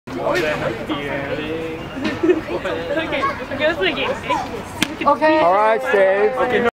Yet another strike out by Stephen the pitcher!
during a baseball game vs. 桃園中壢華勛國小棒球隊 at Taipei American School's Upper Field, playing for TYPA / TAS Off-Season Baseball Team